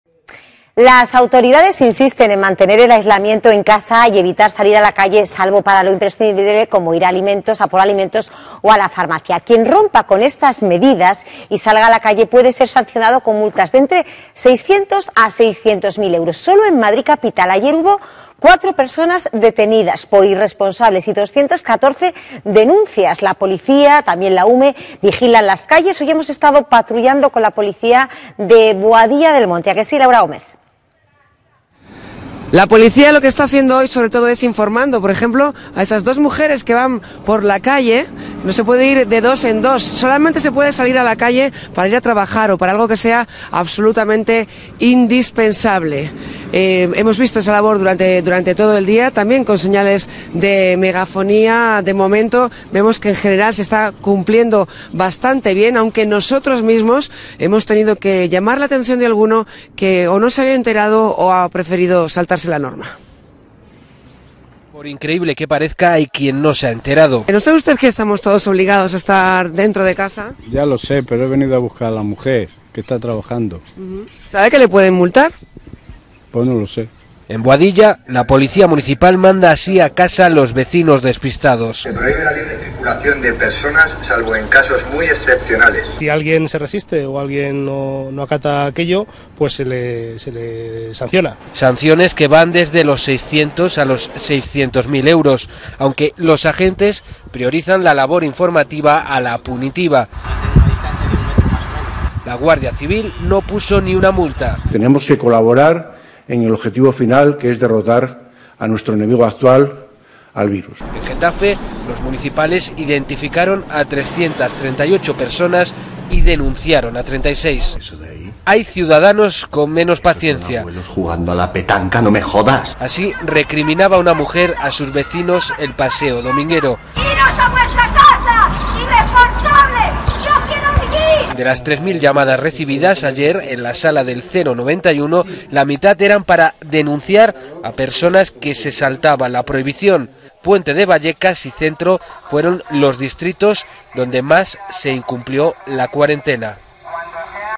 Cuatro detenidos y 214 denuncias por incumplir el aislamiento en Madrid
Las autoridades insisten en mantener el aislamiento en casa y evitar salir a la calle salvo para lo imprescindible, como ir a por alimentos o fármacos. Quien rompa estas medidas y salga a la calle puede ser sancionado con multas de entre 600 y 600.000 euros. Sólo en Madrid capital ayer hubo 4 detenidos y 214 denuncias

Policía, tanto Nacional como Municipal, Guardia Civil y también la Unidad Militar de Emergencias vigilan el cumplimiento en Madrid de las medidas de aislamiento decretadas por el Gobierno en el estado de alarma para combatir la pandemia de coronavirus.

En la primera jornada de vigencia, ayer domingo, tanto la Policía Nacional como la Guardia Civil, realizaron cientos de advertencias pero pocas denuncias y detenciones por incumplir las restricciones de deambular marcadas por el decreto estatal.

Apercibir e informar era la principal misión de las Fuerzas y Cuerpos de Seguridad del Estado (FCSE) en el primer día de entrada en vigor del estado de alarma. Y se han encontrado con algunas personas que pensaban que el decreto del Gobierno no entraba en vigor hasta este lunes o no tenían claros los supuestos en los que sí podían salir a la calle o eran turistas que desconocían todo lo que estaba ocurriendo.

Además, la sala del 091 de la Comunidad de Madrid recibió este domingo cientos de llamadas de vecinos informando de personas que estaban incumpliendo el decreto y también varios episodios de conflictos familiares, generalmente protagonizados por adolescentes que querían salir de casa pese a la advertencia de sus padres.

La Policía Municipal de Madrid, estos días dependiente también del Ministerio de Interior, implemento 199 denuncias este domingo por no cumplir con las restricciones de tránsito e incluso arrestó a una persona.

Centro y Puente de Vallecas fueron los distritos donde más se impusieron, según datos del Ayuntamiento de la capital.

Hoy hemos estado patrullando con la Policía Municipal de Boadilla del Monte.